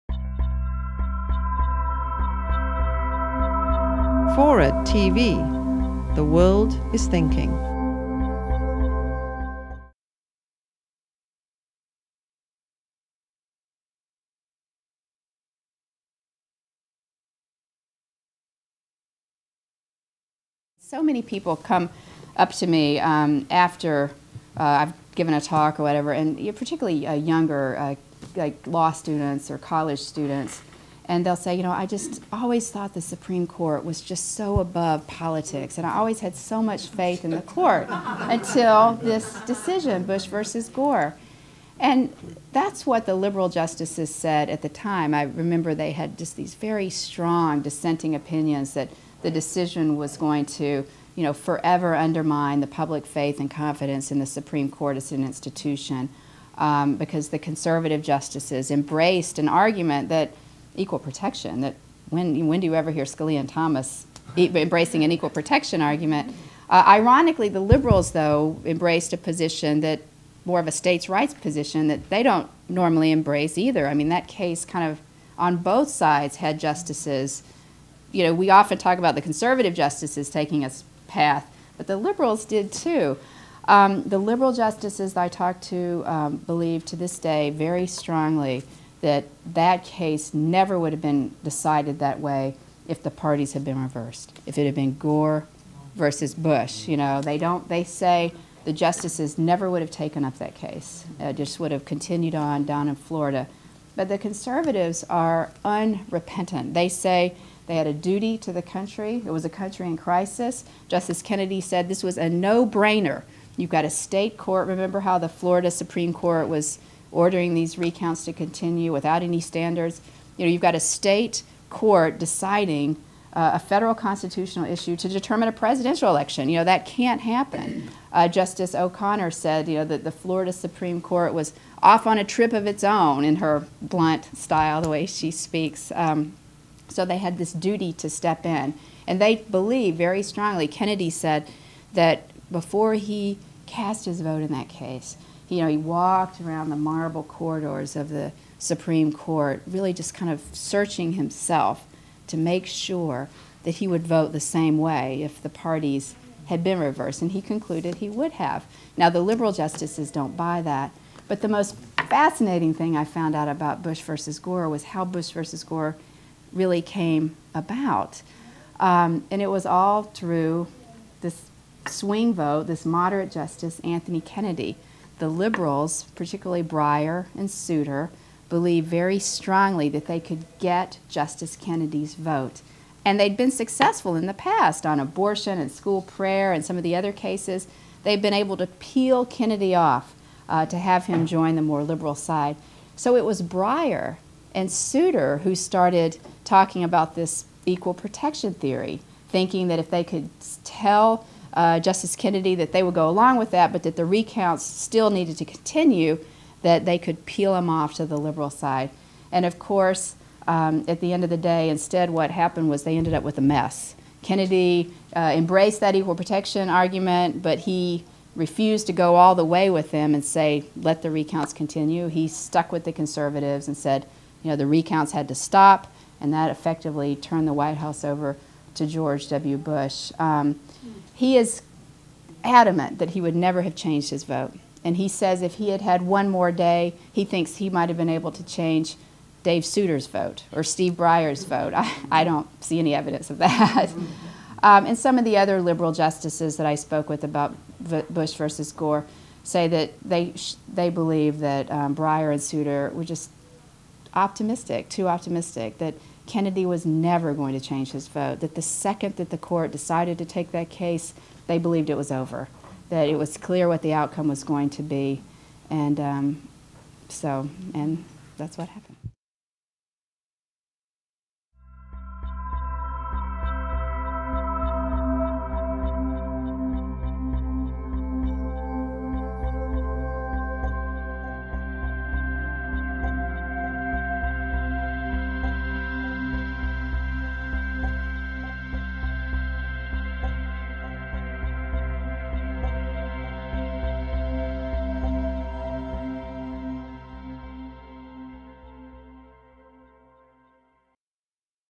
The Supreme Court on Bush vs Gore - Jan Crawford Greenburg
Legal analyst Jan Crawford Greenburg discusses the lasting impact on the U.S. Supreme Court of "Bush v. Gore," the historic decision that determined the outcome of the 2000 presidential election.

Jan Crawford Greenburg talks about "Supreme Conflict: The Inside Story of the Struggle for Control of the United States."

Drawing on unprecedented access to the Supreme Court justices, acclaimed ABC News legal correspondent Jan Greenburg offers an explosive account of the struggle to control the Supreme Court and the direction of the United States.